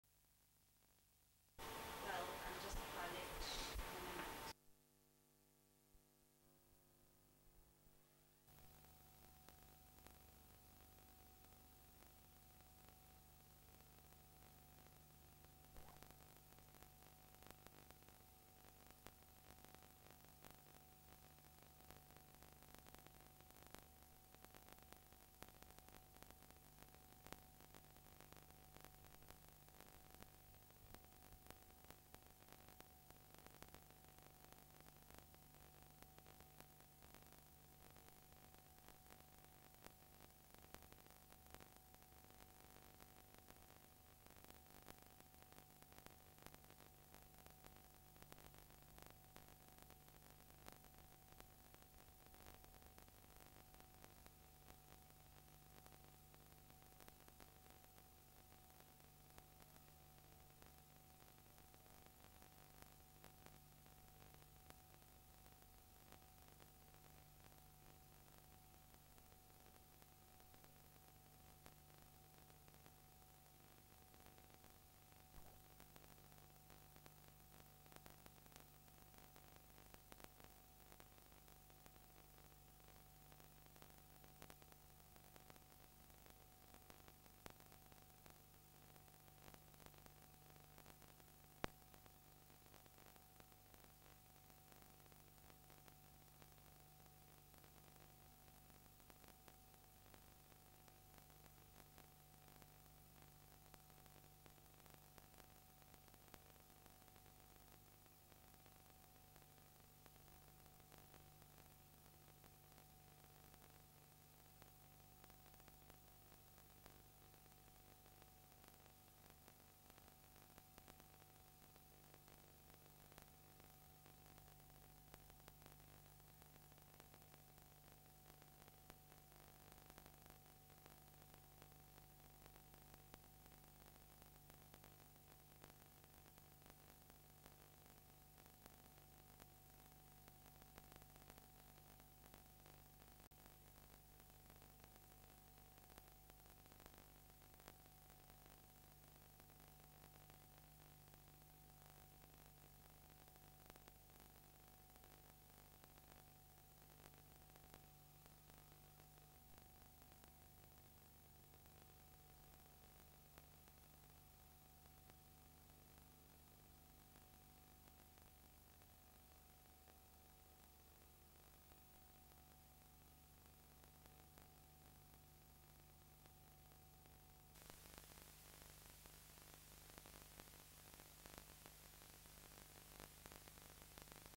John Rubython
John Rubython world renowned photo journalist who had survived most hot spots in the world was settling down to corporate life, had just become a parent and I filmed him just two weeks before his death. He was stabbed to death by a 14 year old burglar in his home in Cape Town, South Africa 1 October 1999.
What happened to his body of work is still unknown.